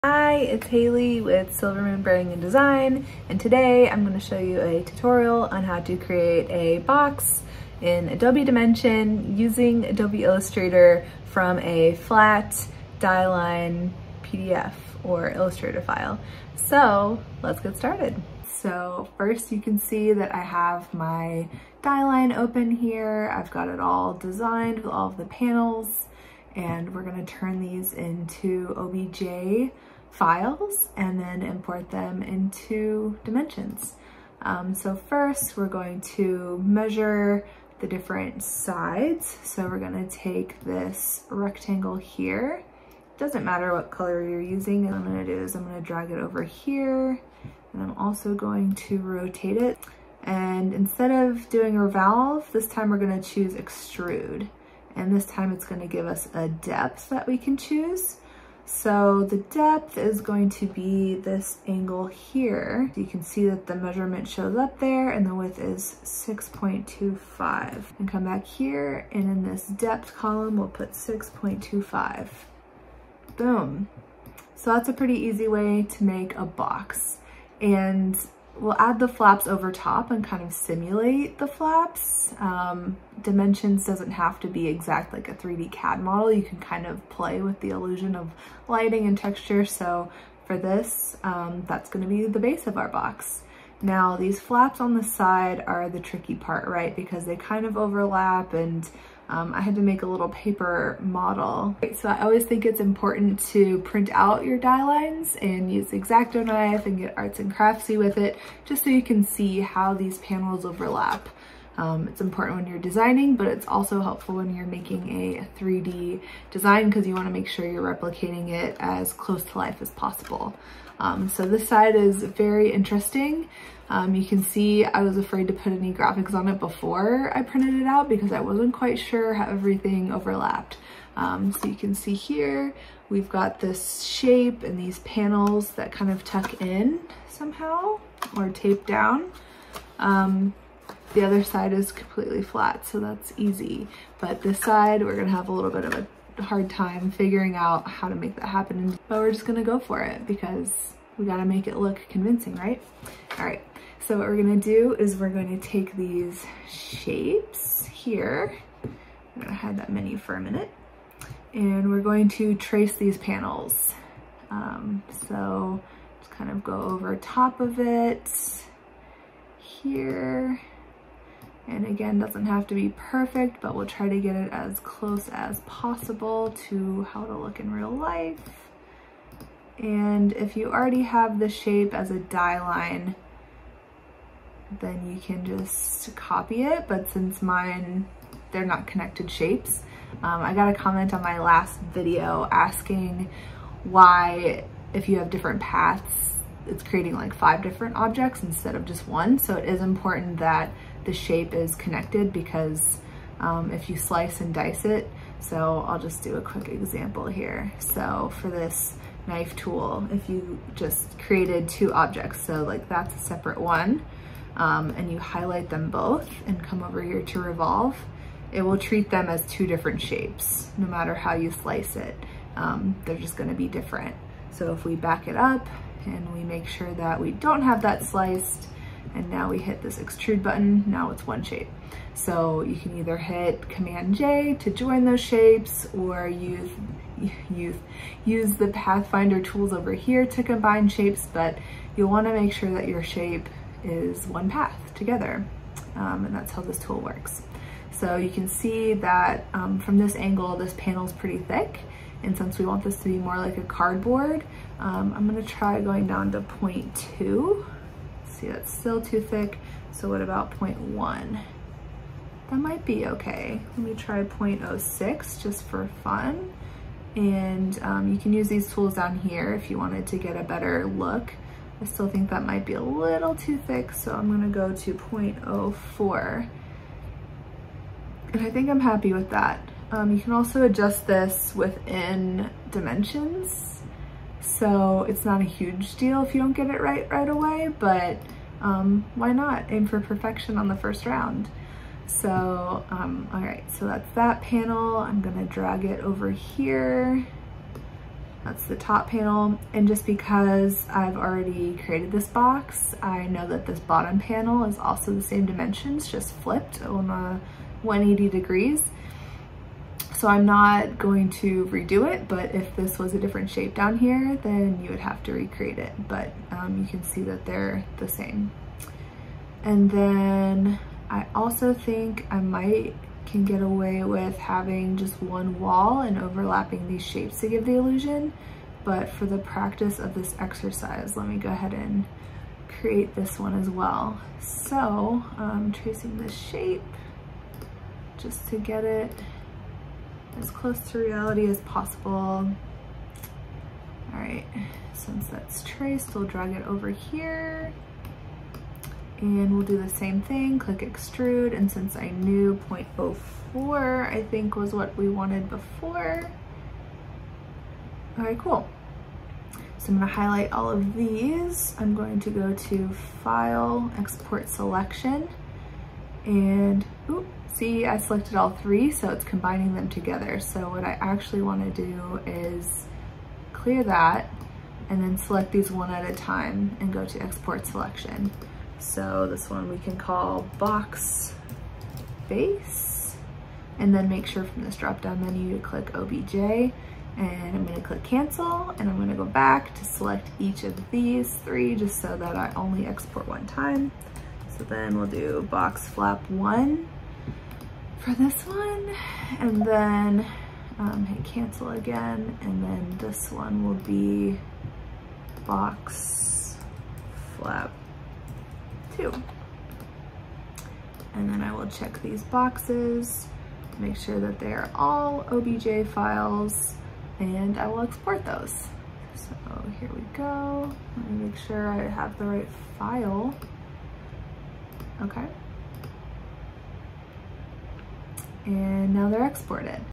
Part 1: How to Design a Custom Box Mockup from Die Lines using Adobe Illustrator and Adobe Dimension
Photoshop 3D no longer working? No worries. You can use Adobe Illustrator's 3D tools to create custom objects to use in your 3D Dimensions mockups.